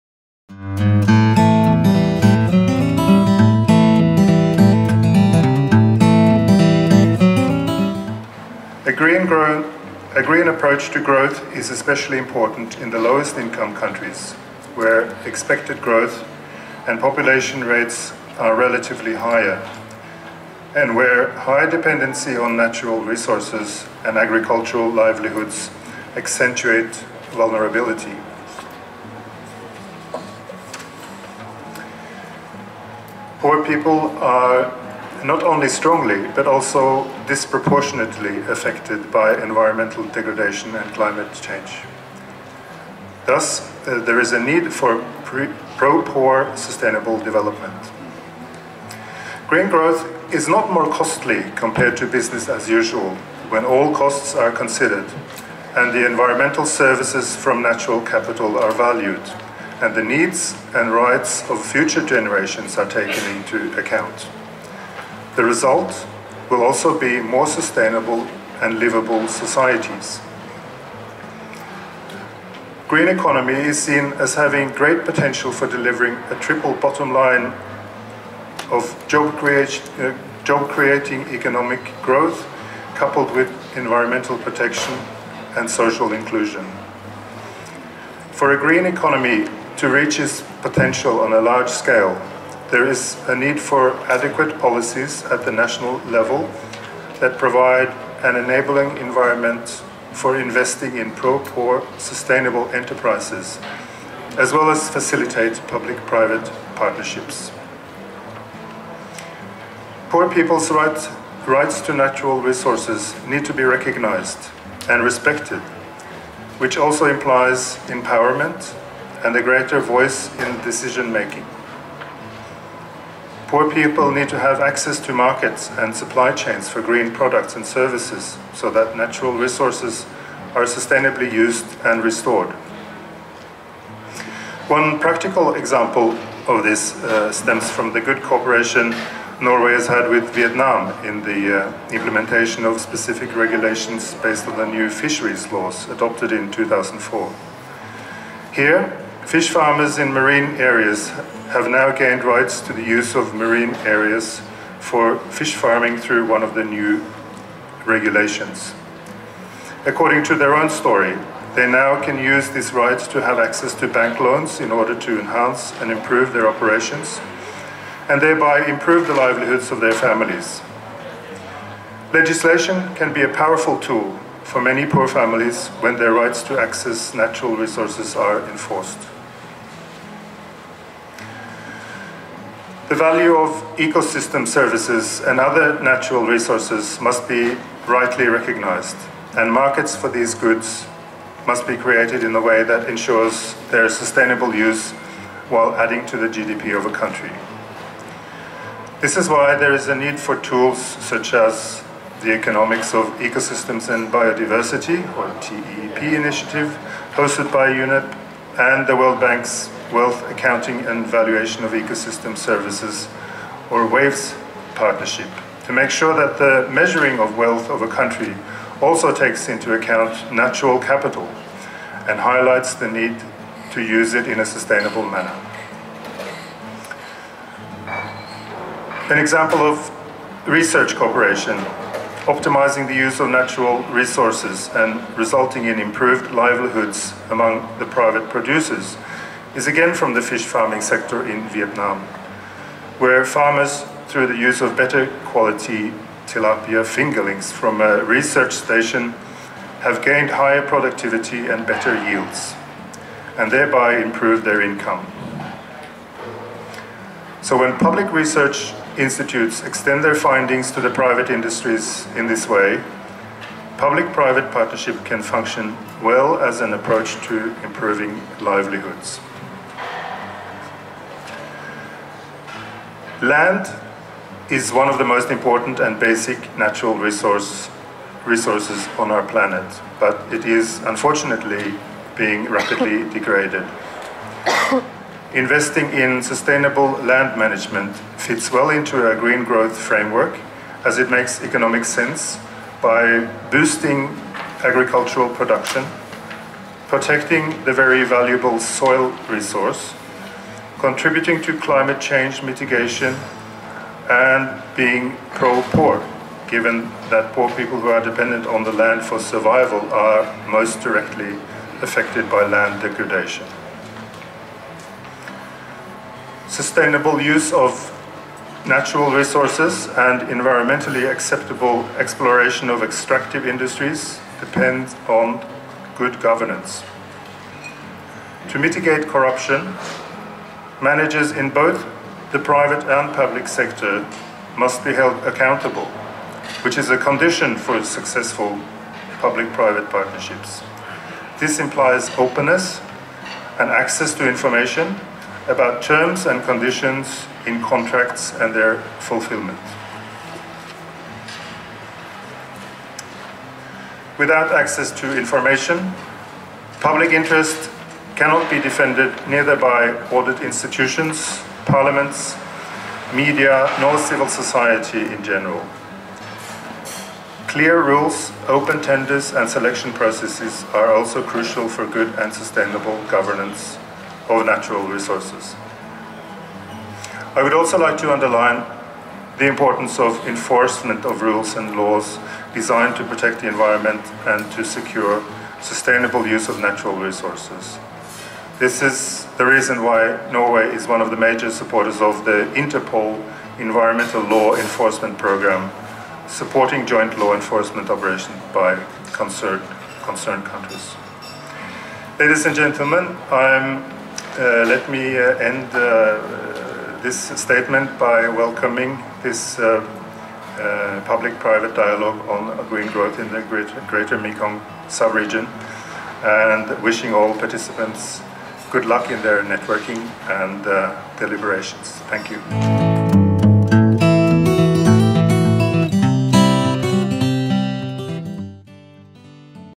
Erik Svedahl, Embassy of Norway - Green Growth in the Greater Mekong Subregion
New opportunities from natural wealth management
A public private dialogue on green growth in the Greater Mekong Subregion
17-20 June 2013 in Bangkok

This session will discuss options for transitioning to a green economy. High level government officials, development partners, investors and business leaders from the Greater Mekong Subregion (GMS) and beyond will participate. An overview of green economy in the GMS region will be given, as well as examples of public and private triple bottom line investments, business practices, innovative financing opportunities and technologies. Options for scaling up success stories and investment opportunities will be debated, including through public-private partnerships and the establishment of enabling conditions. The dialog will provide a forum for sharing lessons and experiences as well as a platform for building networks and joint ventures in the region.